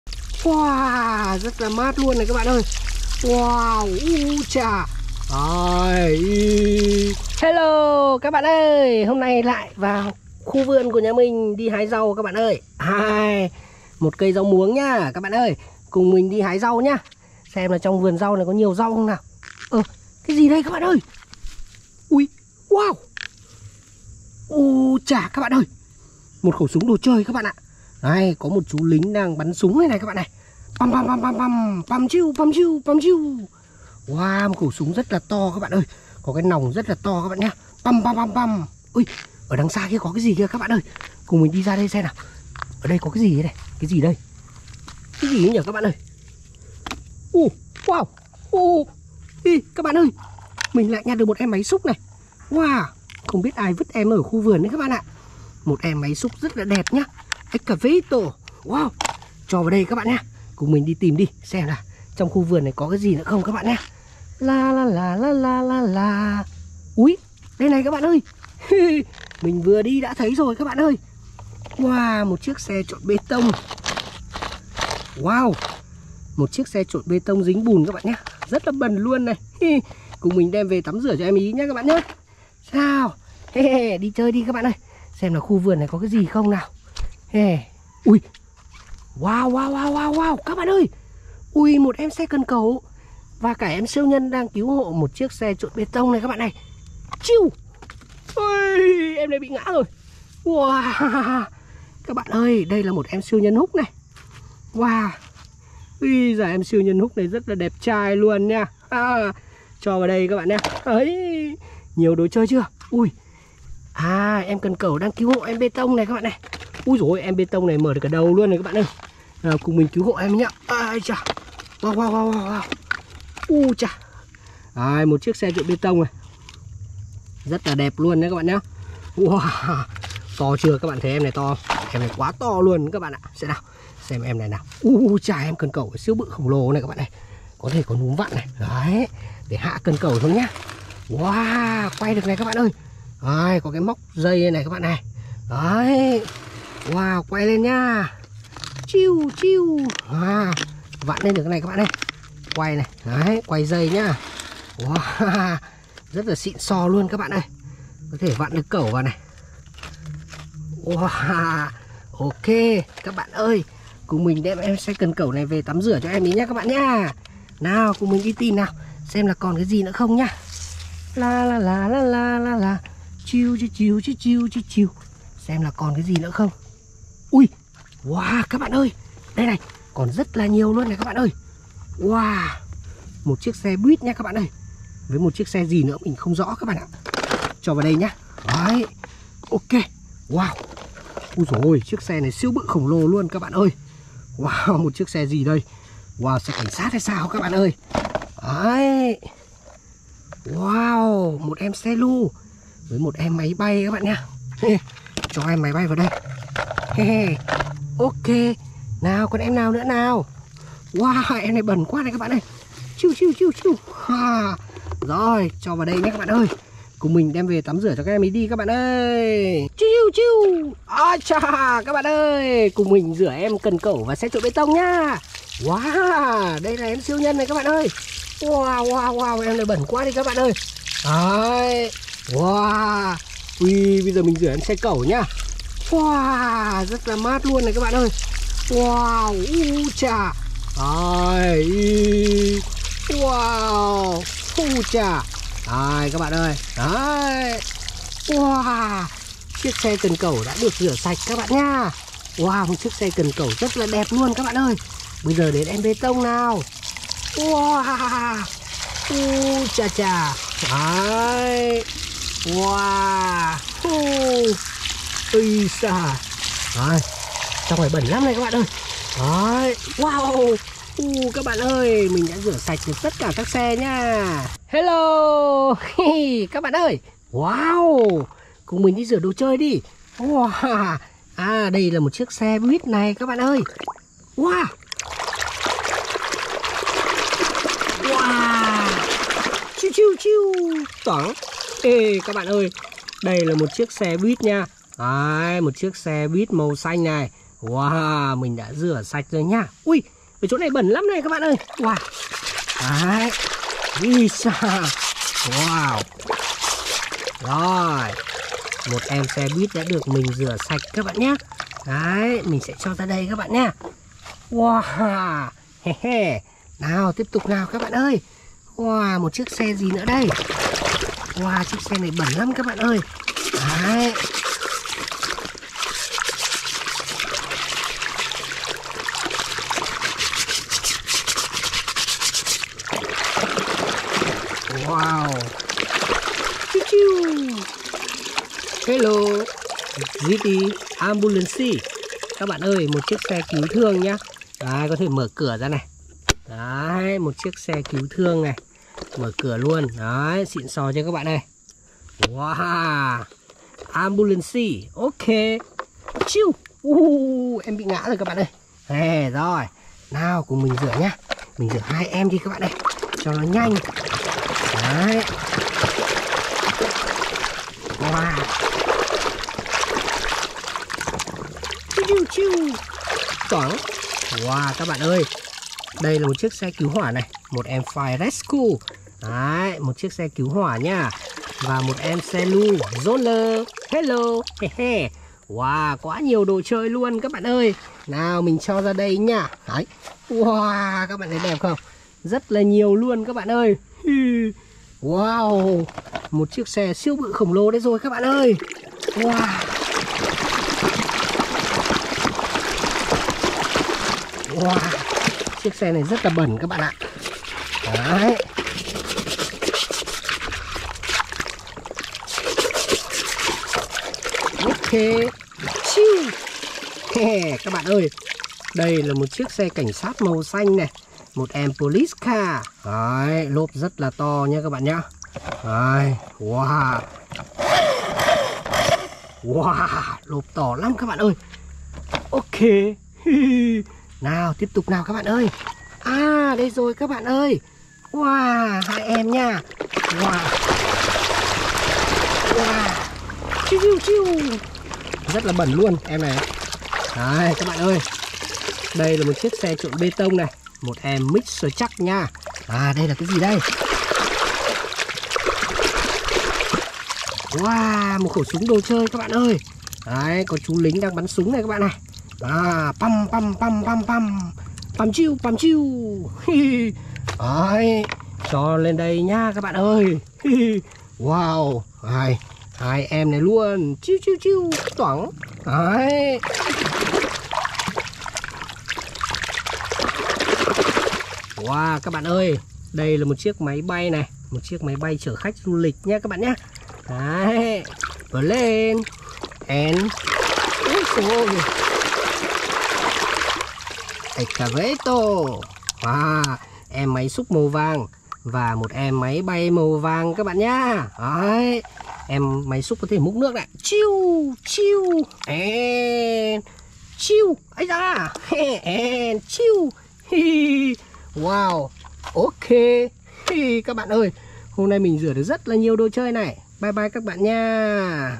Xe Cẩu Cần Dài Cứu Hộ Tìm Xe Trộn Bê Tông Dính Bùn, Máy Xúc Đất, Xe Buýt, Ô Tô Cảnh Sát
Danh Sách Phát Tổng Hợp Các Loại Xe Ô Tô Đồ Chơi TinTin Boys:

© Bản quyền thuộc về TinTin Boys
© Copyright by TinTin Boys Channel ☞ Do not Reup